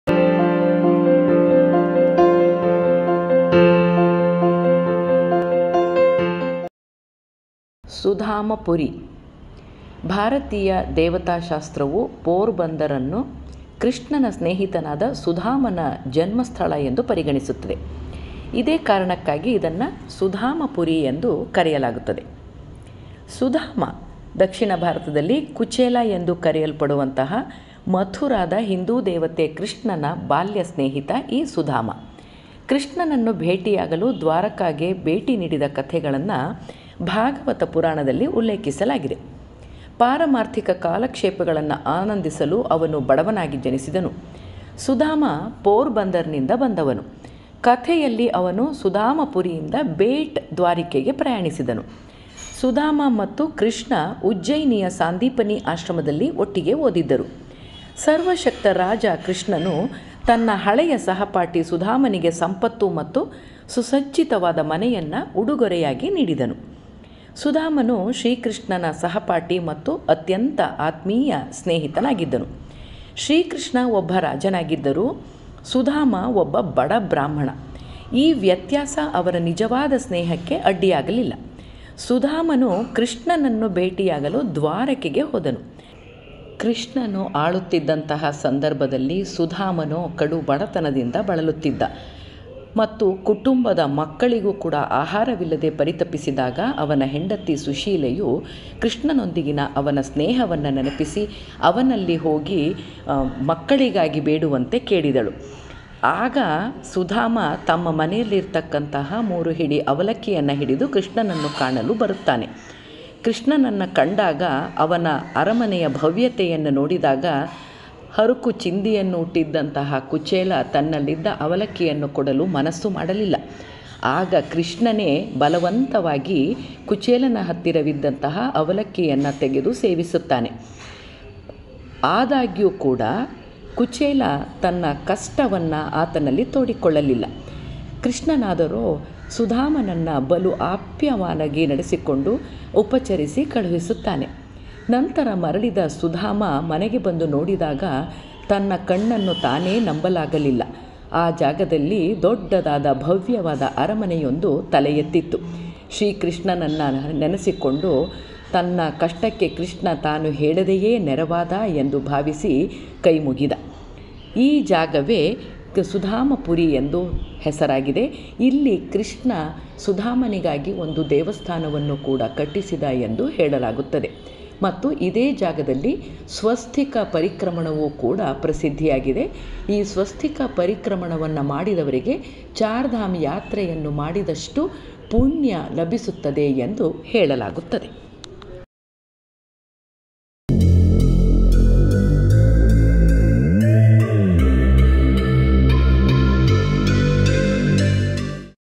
Porbandar ಸುಧಾಮ ಪುರಿ || Tourist Spots || ಸುಂದರ ಪ್ರವಾಸೀ ತಾಣಗಳು
ಭಾರತೀಯ ದೇವತಾ ಶಾಸ್ತ್ರವು ಪೋರ್ಬಂದರ್ ನ್ನು ಕೃಷ್ಣನ ಸ್ನೇಹಿತನಾದ ಸುಧಾಮನ ಜನ್ಮಸ್ಥಳ ಎಂದು ಪರಿಗಣಿಸಲಾಗುತ್ತದೆ ಈ ಕಾರಣಕ್ಕಾಗಿಯೇ ಇದನ್ನು ಸುಧಾಮಪುರಿ ಎಂದು ಕರೆಯಲಾಗುತ್ತದೆ.

ಸುದಾಮ (ದಕ್ಷಿಣ ಭಾರತದಲ್ಲಿ ಕುಚೇಲ ಎಂದೂ ಕರೆಯಲ್ಪಡುವವನು) ಮಥುರಾದ ಹಿಂದೂ ದೇವತೆ ಕೃಷ್ಣನ ಬಾಲ್ಯದ ಸ್ನೇಹಿತ, ಕೃಷ್ಣನನ್ನು ಭೇಟಿಯಾಗಲು ದ್ವಾರಕಾಗೆ ಭೇಟಿ ನೀಡಿದ ಕಥೆಯನ್ನು ಭಾಗವತ ಪುರಾಣದಲ್ಲಿ ಉಲ್ಲೇಖಿಸಲಾಗಿದೆ . ಪಾರಮಾರ್ಥಿಕ ಕಾಲಕ್ಷೇಪಗಳನ್ನು ಆನಂದಿಸಲು ಅವನು ಬಡವನಾಗಿ ಜನಿಸಿದನು. ಸುದಾಮ ಪೋರಬಂದರ್‌ನಿಂದ ಬಂದವನು . ಕಥೆಯಲ್ಲಿ, ಅವರು ಸುದಾಮಪುರಿಯಿಂದ ಬೇಟ್ ದ್ವಾರಕೆಗೆ ಪ್ರಯಾಣಿಸಿದರು. ಸುದಾಮ ಮತ್ತು ಕೃಷ್ಣ ಉಜ್ಜಯಿನಿಯ ಸಾಂದೀಪನಿ ಆಶ್ರಮದಲ್ಲಿ ಒಟ್ಟಿಗೆ ಓದಿದ್ದರು. ಸರ್ವಶಕ್ತ ರಾಜ ಕೃಷ್ಣನು ತನ್ನ ಹಳೆಯ ಸಹಪಾಠಿ ಸುದಾಮನಿಗೆ ಸಂಪತ್ತು ಮತ್ತು ಸುಸಜ್ಜಿತವಾದ ಮನೆಯನ್ನು ಉಡುಗೊರೆಯಾಗಿ ನೀಡಿದನು. [
ಸುದಾಮನು ಶ್ರೀಕೃಷ್ಣನ ಸಹಪಾಠಿ ಮತ್ತು ಅತ್ಯಂತ ಆತ್ಮೀಯ ಸ್ನೇಹಿತನಾಗಿದ್ದನು. ಶ್ರೀಕೃಷ್ಣನು ಒಬ್ಬ ರಾಜನಾಗಿದ್ದನು. ಸುದಾಮ ಒಬ್ಬ ಬಡ ಬಡ ಬ್ರಾಹ್ಮಣ. ಈ ವ್ಯತ್ಯಾಸ ಅವರ ನಿಜವಾದ ಸ್ನೇಹಕ್ಕೆ ಅಡ್ಡಿಯಾಗಲಿಲ್ಲ. ಸುದಾಮನು ಕೃಷ್ಣನನ್ನು ಭೇಟಿಯಾಗಲು ದ್ವಾರಕೆಗೆ ಹೋದನು. ಭಗವಾನ್ ಕೃಷ್ಣನಿಗೆ ಅರ್ಪಿಸಲು ಅವಲಕ್ಕಿಯನ್ನು ಕೊಂಡೊಯ್ಯುತ್ತಾನೆ.
ಇದು ಶ್ರೀ ಕೃಷ್ಣ-ಸುದಾಮರ ನಿಜವಾದ, ಭೌತಿಕವಲ್ಲದ ಸ್ನೇಹಕ್ಕೆ ಅಮರ ಉದಾಹರಣೆಯಾಗಿದೆ. ಇದು ನಿಜವಾದ ಸ್ನೇಹದ ಶಾಶ್ವತ ಸಾಂಕೇತಿಕ ವ್ಯಾಖ್ಯಾನವಾಗಿದೆ. ಅವಲಕ್ಕಿ ಕೃಷ್ಣನಿಗೆ ಬಲುಪ್ರಿಯ
ಕೃಷ್ಣನು ಆಳುತ್ತಿದ್ದಾಗ, ಸುದಾಮನು ಕಡು ಬಡತನದಿಂದ ಬಳಲುತ್ತಿದ್ದನು , ಮತ್ತು ಕುಟುಂಬವು ಮಕ್ಕಳಿಗೆ ಸಹ ಆಹಾರವಿಲ್ಲದೆ ಪರಿತಪಿಸಿದಾಗ, ಅವನ ಹೆಂಡತಿ ಸುಶೀಲೆ ಕೃಷ್ಣನೊಂದಿಗಿನ ಅವನ ಸ್ನೇಹವನ್ನು ನೆನಪಿಸಿದಳು. ಸುದಾಮನು ಶ್ರೀಕೃಷ್ಣನಿಂದ ಯಾವತ್ತೂ ಕೃಪೆಯನ್ನು ಕೇಳಿರಲಿಲ್ಲ. ಅದು ಸ್ನೇಹದ ಉದ್ದೇಶವಲ್ಲ ಎಂದು ಅವರು ಭಾವಿಸಿದರು ಮತ್ತು ಅವರಂತೆ ತಮ್ಮ ಸಾಮರ್ಥ್ಯದಲ್ಲಿ ವಾಸಿಸುತ್ತಿದ್ದರು. ಒಂದು ದಿನ ಕೃಷ್ಣ ಅವರನ್ನು ಭೇಟಿ ಮಾಡಿದ. ಸುದಾಮನು ತನ್ನ ಬಡತನದಿಂದ ನಾಚಿಕೆಪಟ್ಟನು, ಅವನು ಕೃಷ್ಣನನ್ನು ತನ್ನ ಮನೆಗೆ ಆಹ್ವಾನಿಸಲಿಲ್ಲ. ಕೃಷ್ಣನು ಅತಿಥಿಯಾಗಿದ್ದುದರಿಂದ ತಿಂಡಿ ಬಡಿಸಲು ತಮಾಷೆಯಾಗಿ ಕೇಳಿದನು (ಅತಿಥಿಗಳನ್ನು ದೇವರಂತೆ ಪರಿಗಣಿಸಬೇಕು, ಅತಿಥಿ ದೇವೋ ಭವ) ಸುದಾಮನು ತನ್ನ ಬಡತನವನ್ನು ಲೆಕ್ಕಿಸದೆ ಒಳಗೆ ಹೋಗಿ ಸ್ವಲ್ಪ ಕಡಲೆ ಕಾಳುಗಳನ್ನು ಪಡೆದನು (ಎರಡೂ ಅವರೆಕಾಳು ಕೃಷ್ಣನಿಗೆ ಪ್ರಿಯವಾದದ್ದು ಮತ್ತು ಮನೆಯಲ್ಲಿ ಬೇರೇನೂ ಇಲ್ಲದ ಕಾರಣ). ಕೃಷ್ಣನು ಆ ತಿಂಡಿಯನ್ನು ರುಚಿಯಾಗಿ ತಿಂದು ಹಿತವಾಗಿ ಹೊರಟನು. ಸುದಾಮನು ಮನೆಯೊಳಗೆ ಹೋಗಲು ಹಿಂತಿರುಗಿದಾಗ, ಅವನು ಆಶ್ಚರ್ಯಚಕಿತನಾದನು, ಅವನ ಮನೆಯು ಒಂದು ಗುಡಿಸಲಿನಿಂದ ಅರಮನೆಗೆ ಬದಲಾಗಿದೆ. ಅವರ ಕುಟುಂಬವು ಶ್ರೀಮಂತ ವೇಷಭೂಷಣಗಳನ್ನು ಧರಿಸಿ ಅವನಿಗಾಗಿ ಕಾಯುತ್ತಿರುವುದನ್ನು ಅವನು ಕಂಡುಕೊಂಡನು.
ಸುದಾಮನು ವಿವರಣೆಯನ್ನು ಕೇಳಲು ಕೃಷ್ಣನ ಬಳಿಗೆ ಹೋದನು (ಮತ್ತೆ ತನ್ನ ಸ್ನೇಹಿತನಿಗೆ ಕಡಲೆ ಕಾಳುಗಳನ್ನು ಉಡುಗೊರೆಯಾಗಿ ತೆಗೆದುಕೊಂಡನು). ಕೃಷ್ಣನು ತನ್ನ ಹಳೆಯ ಸ್ನೇಹಿತನನ್ನು ನೋಡಿ ಬಹಳ ಸಂತೋಷಪಟ್ಟನು. ಅವನು ಅವನನ್ನು ರಾಜಮನೆತನದಿಂದ ಮತ್ತು ಪ್ರೀತಿಯಿಂದ ನಡೆಸಿಕೊಂಡನು. ಇದೆಲ್ಲದರಿಂದ ಮುಳುಗಿದ ಸುದಾಮನು ಅಳುತ್ತಾನೆ ಮತ್ತು ಕೃಷ್ಣನು ಹೇಳುತ್ತಾನೆ, "ನೀವು ಯಾವಾಗಲೂ ನನಗೆ ನೀಡುತ್ತಿರುವ ಕಡಲೆ ಕಾಳುಗಳನ್ನು ನಾನು ಪ್ರೀತಿಸುತ್ತೇನೆ. ನನಗೆ ಬೇರೇನೂ ಬೇಕಾಗಿಲ್ಲ." (ಇದು ಮನೆಯಲ್ಲಿ ಯಾರಾದರೂ ಊಟ ಮಾಡುವ ಮೊದಲು ದೇವರಿಗೆ ಅನ್ನವನ್ನು ಅರ್ಪಿಸುವ ಆಚರಣೆಯನ್ನು ಹುಟ್ಟುಹಾಕಿತು - ಸ್ವಲ್ಪಮಟ್ಟಿಗೆ ಊಟದ ಮೊದಲು ಕೃಪೆ ಹೇಳುವ ಪದ್ಧತಿಗೆ ಹೋಲುತ್ತದೆ). ಸುದಾಮನು ಹೊರಟುಹೋದಾಗ, ಕೃಷ್ಣನು ಅರಮನೆಯಲ್ಲಿ ಎಲ್ಲರಿಗೂ ವಿವರಿಸಿದನು, "ನಾನು ಅವನಿಗೆ ಕೊಟ್ಟದ್ದೆಲ್ಲವೂ ಅವನ ಭಕ್ತಿಯಿಂದ ಮಾತ್ರ". ಹೀಗೆ ಶ್ರೀಕೃಷ್ಣನು ತನ್ನ ಭಕ್ತನಬಗ್ಗೆ ಕಾಳಜಿ ವಹಿಸುತ್ತಾನೆ . ಹಿಂದಿರುಗುವ ಪ್ರಯಾಣದಲ್ಲಿ, ಸುದಾಮನು ತನ್ನ ಪರಿಸ್ಥಿತಿಗಳನ್ನು ಆಲೋಚಿಸಿದನು ಮತ್ತು ತನಗಿದ್ದ ಉತ್ತಮ ಸ್ನೇಹಿತನಿಗೆ ಕೃತಜ್ಞನಾಗಿದ್ದನು,, ಮತ್ತು ಅದರ ನಂತರ ಕಠಿಣ ಜೀವನವನ್ನು ನಡೆಸಿದರು, ಯಾವಾಗಲೂ ಭಗವಂತನಿಗೆ ಕೃತಜ್ಞರಾಗಿರುತ್ತಿದ್ದರು. ದ್ವಾರಕಾದಲ್ಲಿ ಭಗವಾನ್ ಕೃಷ್ಣ ಮತ್ತು ಸುದಾಮನ ನಂಬಿಕೆ ಮತ್ತು ಸ್ನೇಹದ ಈ ಪವಾಡವು ಅಕ್ಷಯ ತೃತೀಯ ಹಬ್ಬದ ಆಚರಣೆಯೊಂದಿಗೆ ಸಂಬಂಧಿಸಿದೆ .
Sudama Temple is one of the revered sites of Gujarat. This temple is dedicated to Sudama who was the childhood friend of Lord Krishna. This temple is often visited by thousands of devotees particularly the newly married Rajasthani Kshatriya couples who visit the temple to take the blessing. Located at the centre of Porbandar, it is one exceptional temple in India which is dedicated to this great devotee of Lord Krishna.
Sudama Temple was constructed in 1902 and 1907 at the centre of the city. It is one of the historically significant sites in Gujarat. The construction of the temple was hampered to some extend by the depletion of the funds. As the money was less it was raised by collecting donations and organizing dramas to raise the fund. All the rich traders contributed funds which can be used in the construction of the temple. Though the architecture is not too lavish yet it is one of the well-built temples of Porbandar which is often visited by thousands of tourists and devotees. Built with white marble this temple has a number of carved pillars which decorate the temple, open from all sides this temple has a shikhara which is decorated with splendid architecture and carvings. These carvings are also visible above the pillars and the arches which adjoin the pillars. With such architecture this temple is dedicated to the shrine of Sudama which is built in simple structure.

Date of Visit : 3Rd December 2021